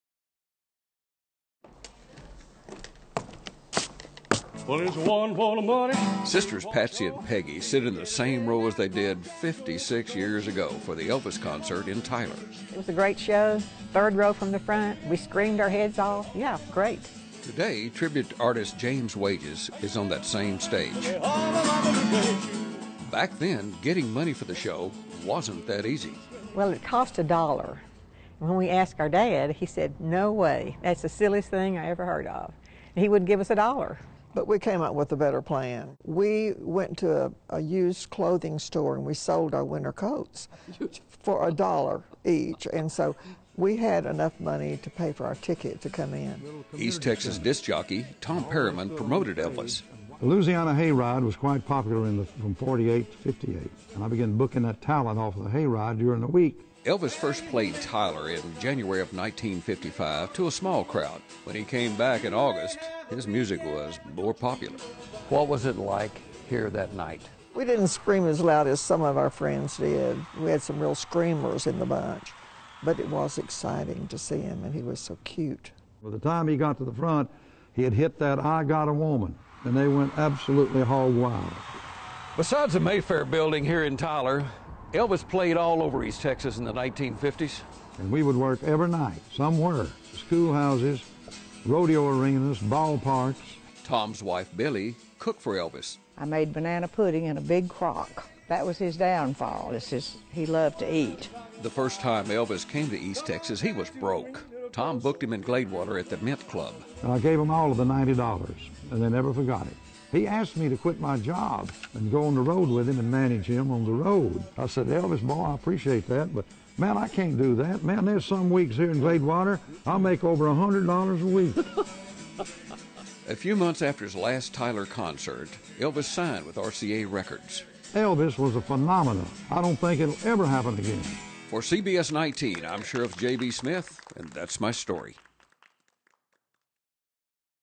JB's Journal - Elvis in Tyler
This week, Elvis fans remembered the King of Rock n Roll on what would have been his 77th birthday. Tonight on J.B.'s Journals, we'll show you how Elvis got his start in East Texas.
From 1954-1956, Elvis played many concerts throughout Texas. Elvis is quoted as telling a Dallas Morning News reporter that he owed a lot to Texas; saying, they are the ones who put me over the top.